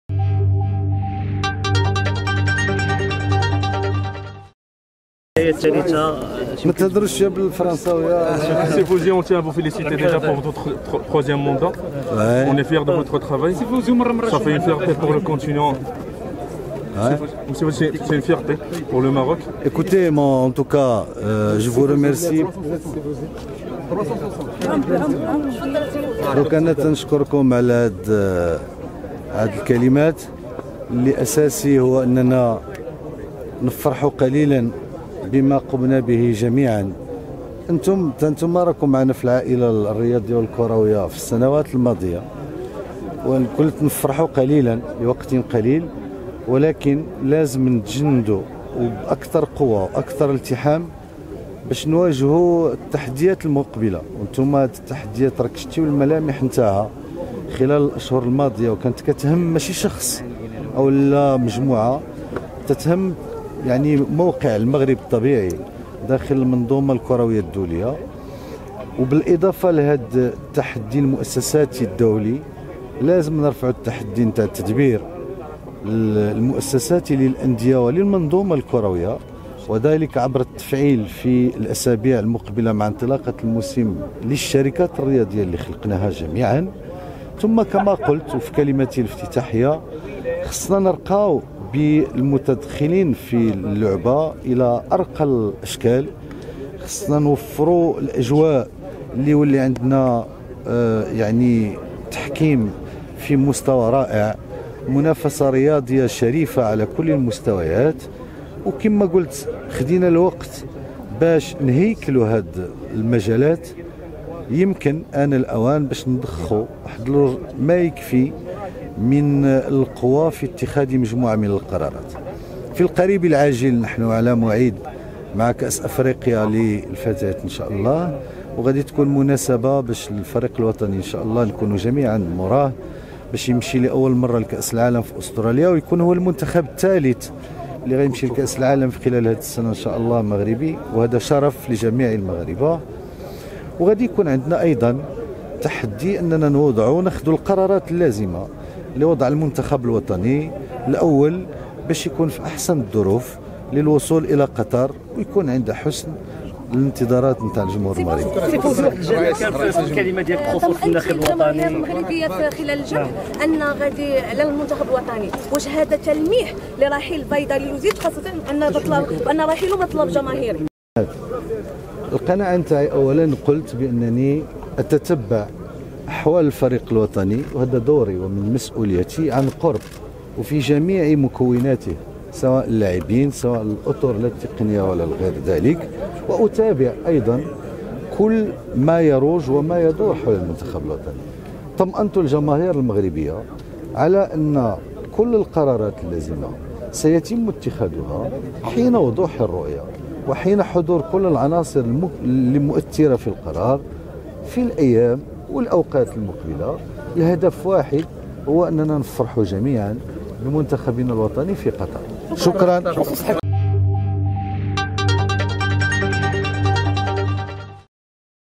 لقجع بعد إعادة انتخابه رئيساً للجامعة:"أتابع أحوال المنتخب وسيتم إتخاذ قرارات جديدة قبل مونديال قطر"
تم اليوم الجمعة إعادة انتخاب فوزي لقجع رئيسا للجماعة الملكية المغربية لكرة القدم لولاية ثالثة.